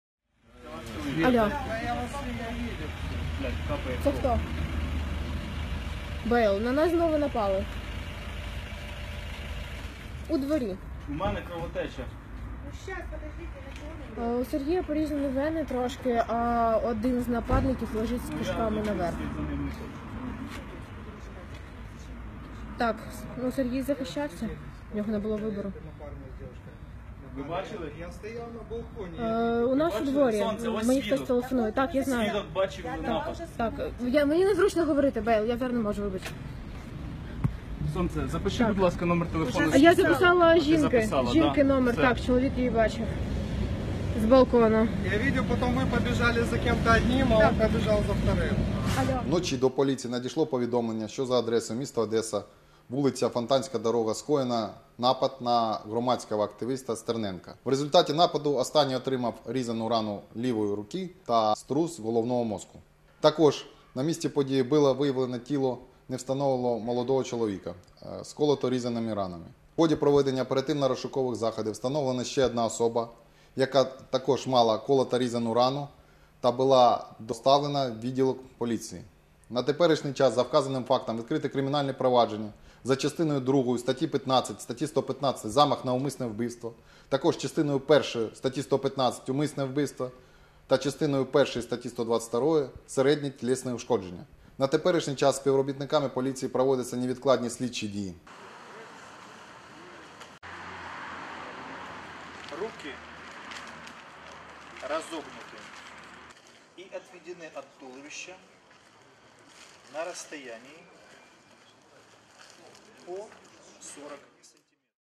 Полиция расследует обстоятельства нападения на Сергея Стерненко
24 мая, ночью в полицию поступило сообщение о том, что на улице Фонтанская дорога совершено нападение на Сергея Стерненко, в результате которого он был ранен и позже доставлен в больницу с сотрясением головного мозга и резаной раной руки.
На месте происшествия полицейские обнаружили тело мужчины с колото-резаными ранами брюшной полости.
Также установлено еще одного участника события с колото-резаными ранениями, которого для дачи объяснений доставили в полицию.
Указанное событие квалифицировано по ч.1 ст.122 (умышленное средней тяжести телесное повреждение), ч.1 ст. 115 (покушение на убийство) и ч.1 ст. 115 (умышленное убийство) Уголовного кодекса Украины.

Правоохранители устанавливают обстоятельства происшествия и лиц, к ней причастны. Продолжается следствие.